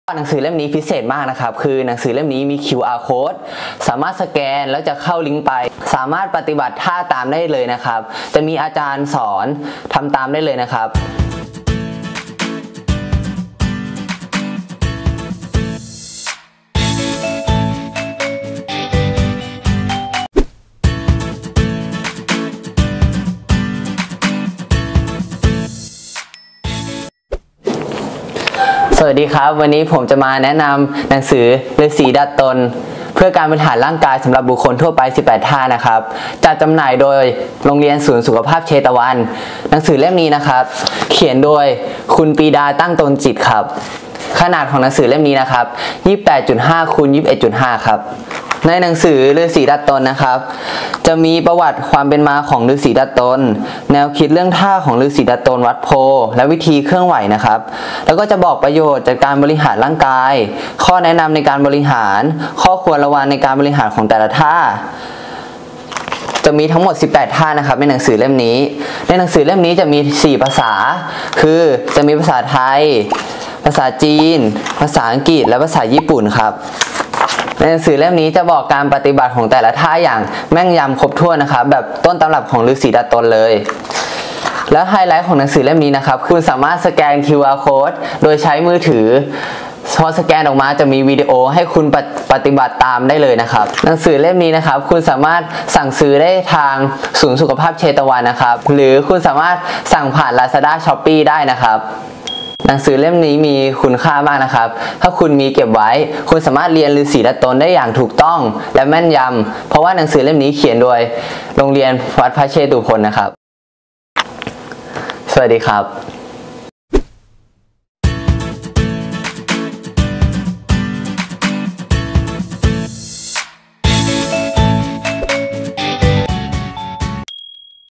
หนังสือ "ฤาษีดัดตน"โฉมใหม่ Scan QR Code ดูท่าได้ มีจำหน่ายแล้ว
หนังสือตำราเรียน "ฤาษีดัดตน"สแกน ดูภาพเคลื่อนไหวได้ ง่ายต่อการฝึกหัด ปฏิบัติ ได้ง่าย